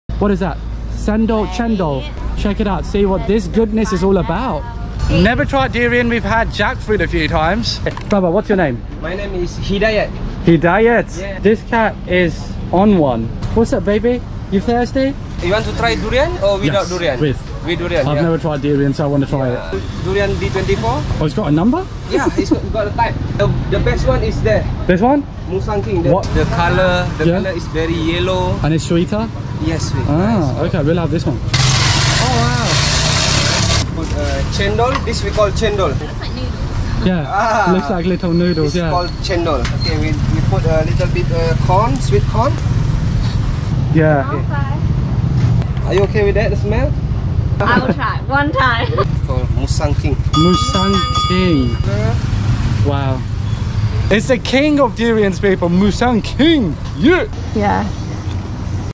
FIRST TIME trying Durian in Kuala Lumpur, Malaysia 🇲🇾
Join us on a mouthwatering adventure as we try the infamous durian fruit for the very first time in Kuala Lumpur, Malaysia! Known for its pungent smell and creamy texture, this exotic fruit is loved by some and despised by others. Watch as we explore the bustling streets of Kuala Lumpur to find the best durian vendor and sample this unique delicacy. Don't miss out on this exciting culinary experience!